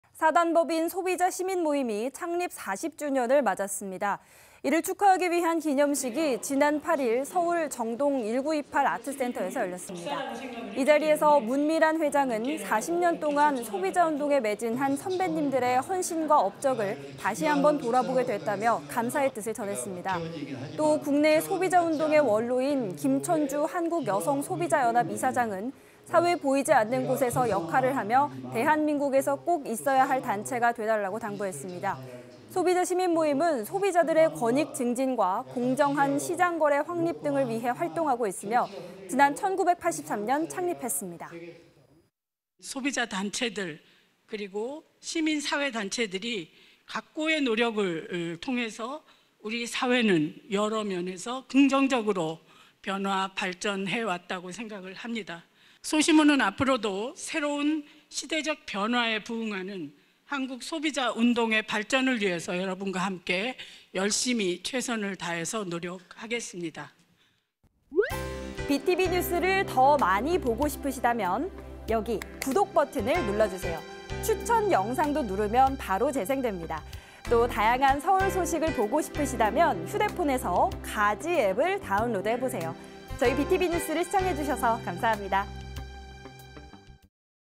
(사)소비자시민모임, 창립 40주년 기념식 개최_SK broadband 서울뉴스
[B tv 서울뉴스 김대우 기자]

[기사내용]
(사)소비자시민모임이 창립 40주년을 맞았습니다.

이를 축하하기 위한 기념식이
지난 8일, 서울 '정동 1928 아트센터'에서 열렸습니다.

이 자리에서 문미란 회장은
"40년 동안 소비자운동에 매진한
선배님들의 헌신과 업적을
다시 한번 돌아보게 됐다"며
감사의 뜻을 전했습니다.

또 국내 소비자운동의 원로인
김천주 한국여성소비자연합 이사장은
"사회 보이지 않는 곳에서 역할을 하며
대한민국에서 꼭 있어야 할 단체가
돼달라"고 당부했습니다.

소비자시민모임은 소비자들의 권익 증진과
공정한 시장 거래 확립 등을 위해 활동하고 있으며,
지난 1983년 창립했습니다.

[ 문미란 / (사)소비자시민모임 회장]
"소비자단체들, 그리고 시민사회단체들이
각고의 노력을 통해서
우리 사회는 여러 면에서 긍정적으로
변화·발전해 왔다고 생각합니다.

소비자시민모임은 앞으로도 새로운 시대적 변화에
부응하는 한국 소비자운동의 발전을 위해서
여러분과 함께 열심히 최선을 다해서 노력하겠습니다."

(촬영편집ㅣ김웅수 기자)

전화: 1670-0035